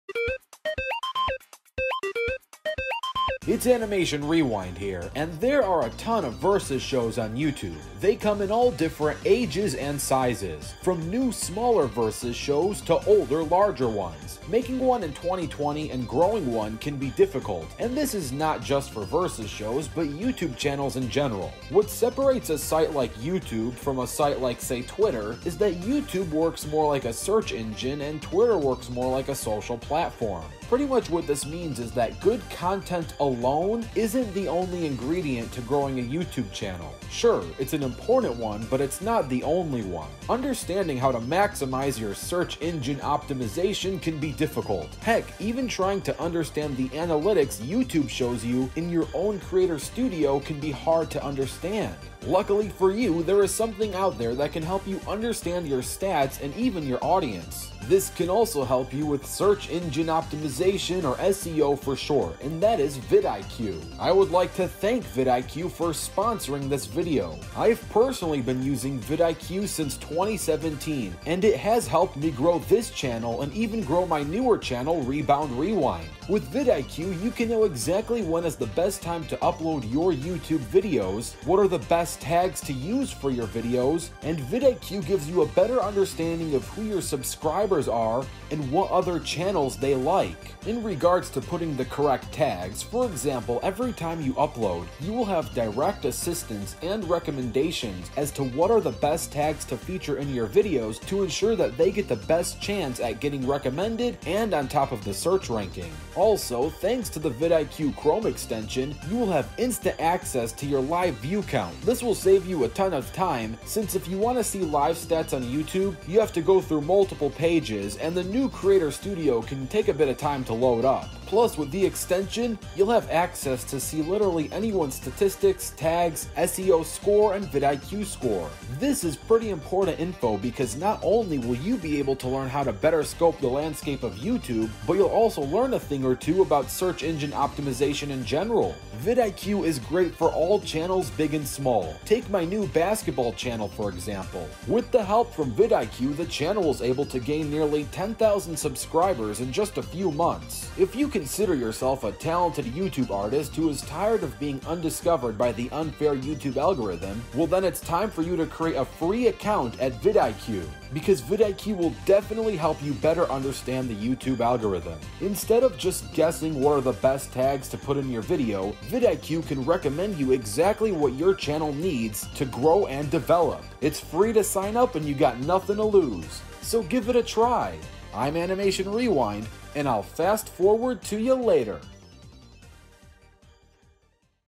How to grow your VS Series/YouTube channel with vidIQ!
In this video I go over how vidIQ can help you understand YouTube Search Engine Optimization (SEO), and how vidIQ can help grow your YouTube channel.